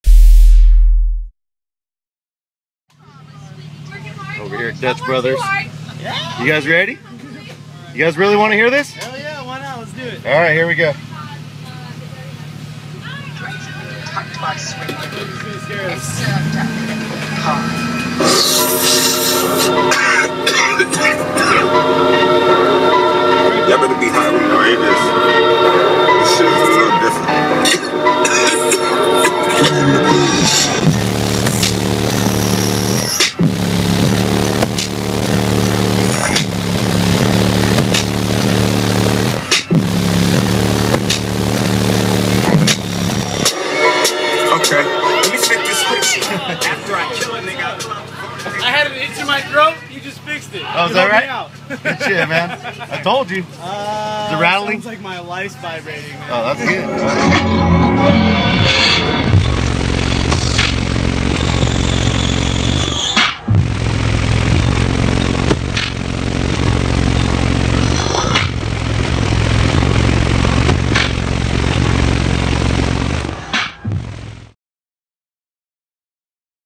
Life Vibrating, Kiosk Crushing BASS - Dutch Bros vs 30,000 Watt Sound System
Dutch Bro's is my favorite coffee. I go there probably once a day, if not every other day. Today, i came rolling up in the Tahoe. They have not seen or heard it yet. Today, i change that. LOL

This current system consists of:

2001 SMD Chevy Tahoe
Mac Mini / 10.2" Touchscreen
Pioneer P710 Bluetooth Headunit
4 18" Ascendant Audio SMD Subwoofers
10 Rockford Fosgate 6.5"
2 Rockford Fosgate 8"
2 Rockford Fosgate 4"
12 Rockford Fosgate 1"
8 Rockford Fosgate T2500.1 bdCP (Bass) (2 amps on each sub)
2 Rockford Fosgate T1000.4 (Midbass/Midrange)
1 Rockford Fosgate T600.2 (Tweeters)
2 Rockford Fosgate 3sixty.2 Processors
4 350 Amp Mechman Alternators
10 XS Power D3100 Batteries
Second Skin Sound Deadened Top to Bottom
Viper 5901 Security
Ridetech Shockwave Titanium Suspension
Wilwood 6 piston 16" Caliper Brakes
DJM and Spohn Control Arms
"Meadecherry" Paint by Roy Devore
.and more